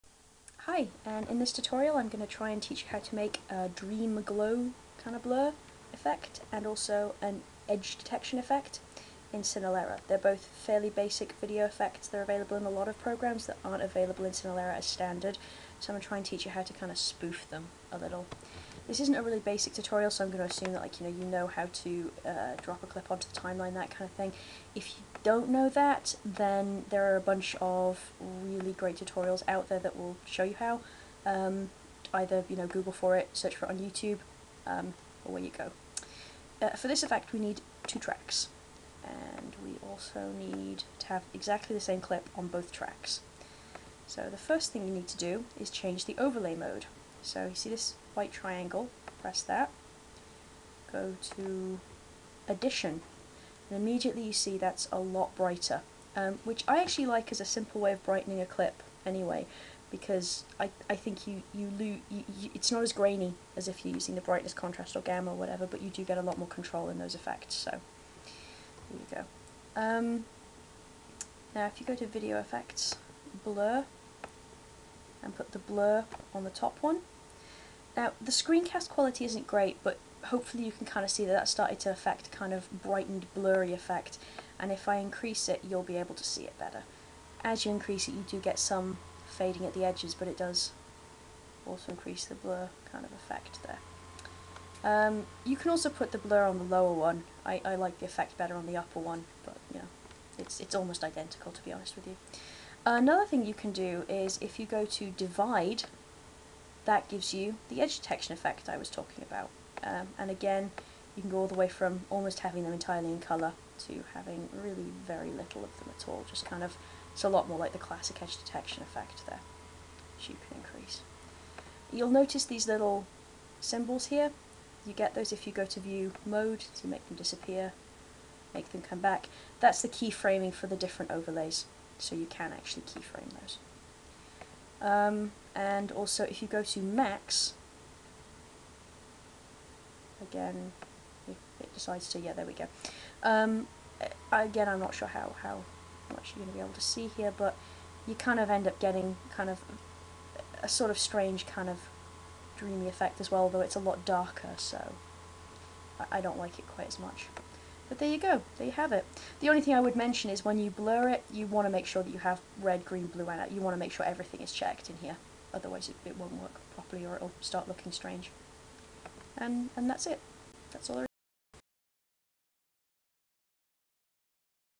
Cinelerra Tutorial: Dream Glow & Edge Detection effect
A tutorial explaining how to spoof an edge detection and dream glow type of effect in cinelerra.  Made because I couldn't find any other video tutorials explaining this, so I figured I'd contribute one.